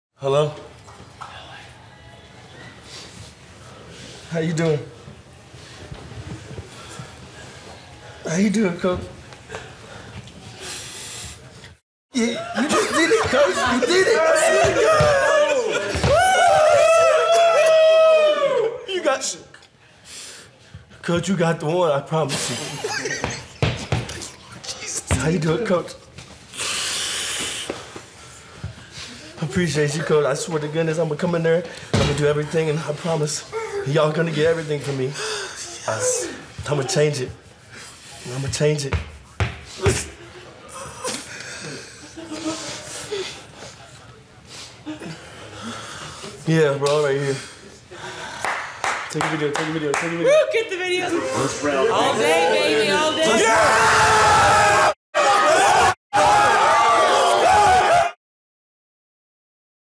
Kyren Williams is drafted to the Los Angeles Rams
Alongside his family, Kyren Williams gets the call from the LA Rams to go Round 5, Pick 164 in the 2022 NFL Draft